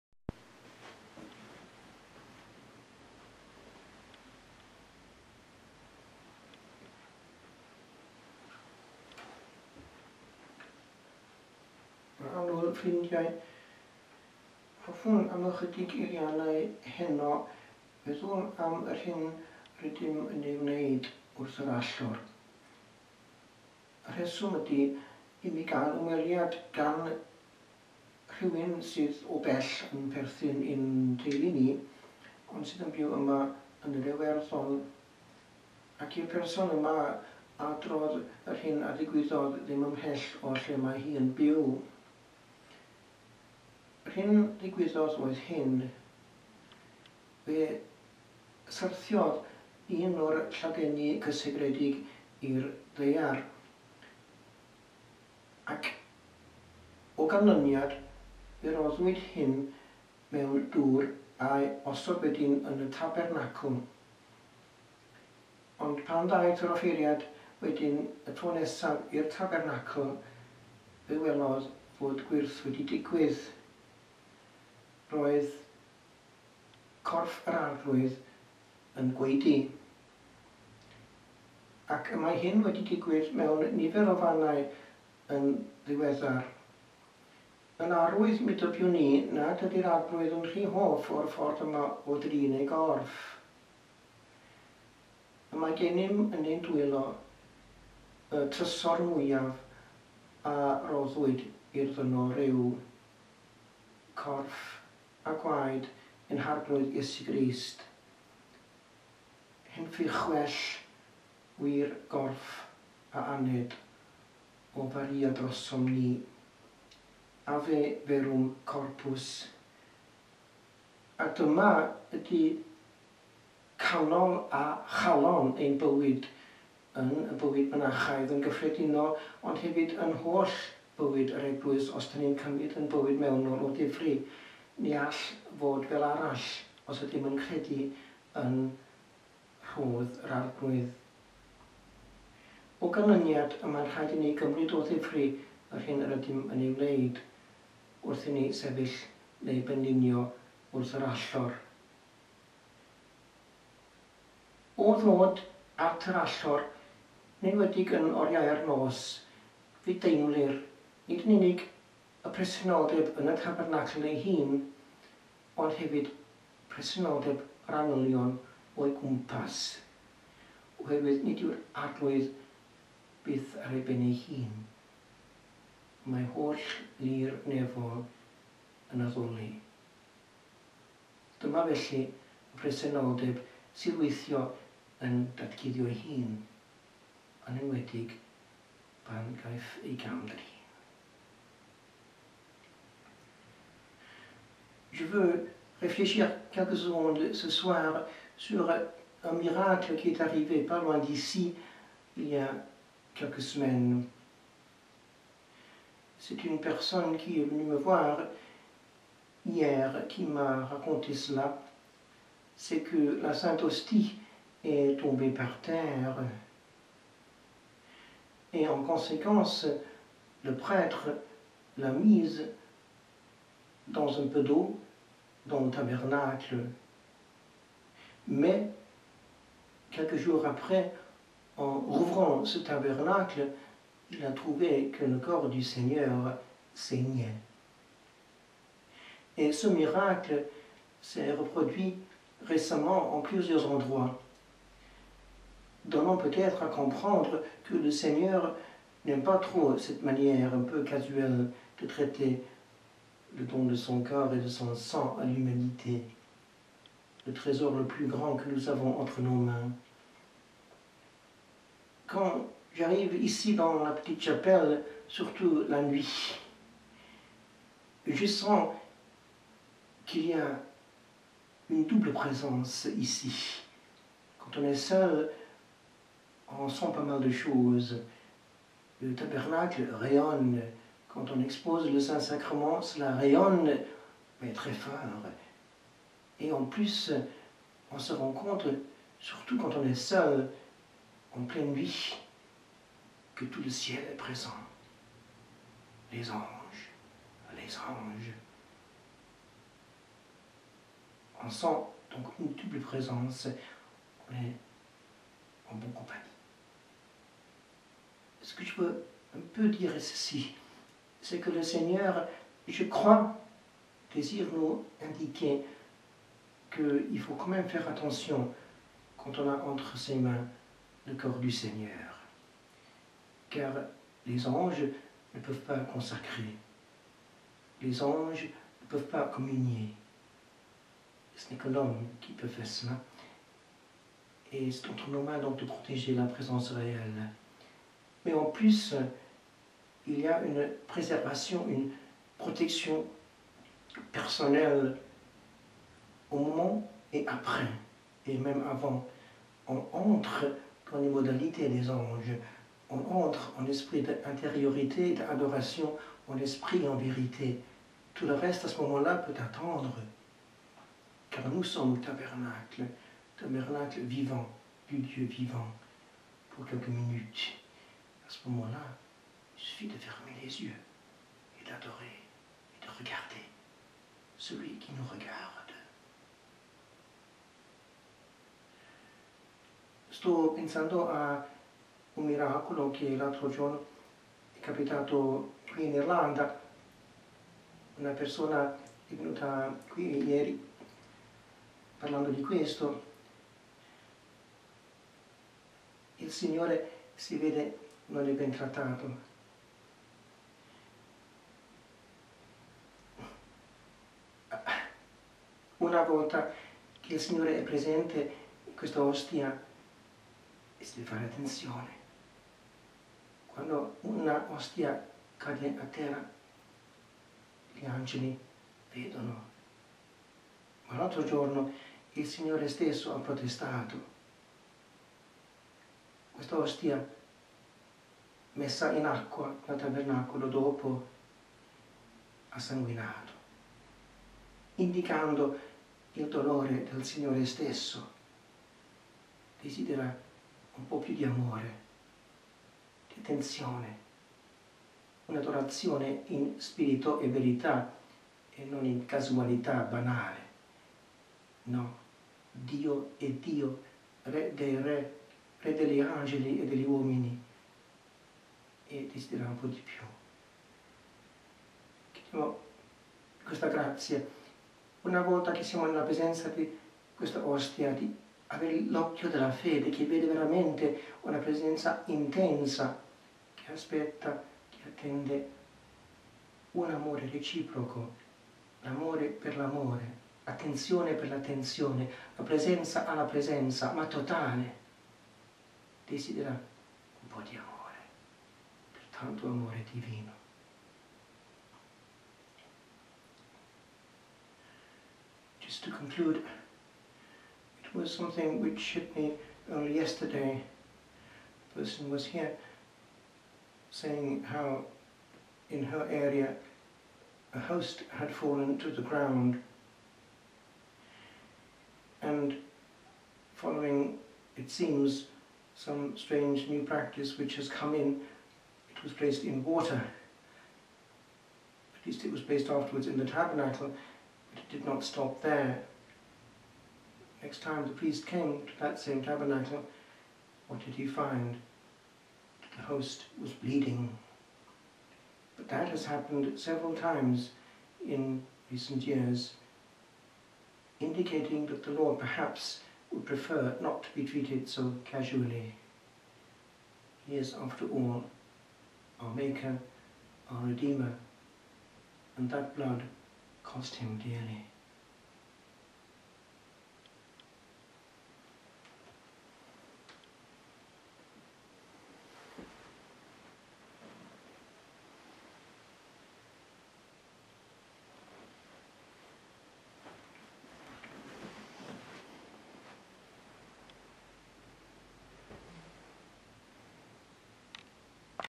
Fr. David Jones - 2nd February 2013, Multilingual Eucharistic homily (Pregeth Ewcharistaidd)
Fr. David Jones - 2nd February 2013, Multilingual homily